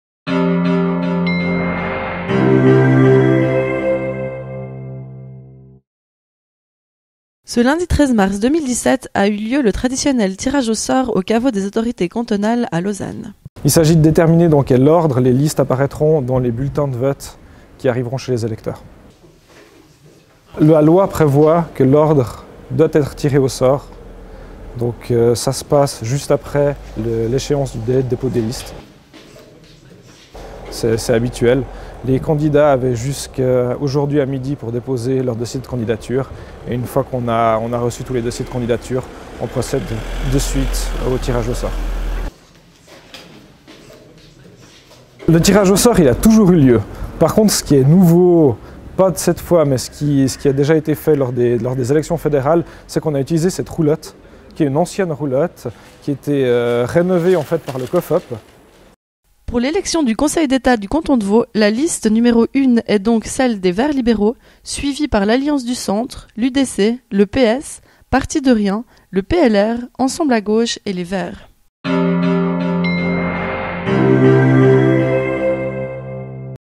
Election au Conseil d'Etat: l'ordre des listes des partis tiré au sort
Lundi 13 mars 2017 a eu lieu le traditionnel tirage au sort des listes des partis pour l'élection au Conseil d'Etat. Après l’échéance pour le dépôt des listes lundi à midi, le hasard a donc attribué l’ordre des listes qui figurera dans le cahier de bulletins de vote.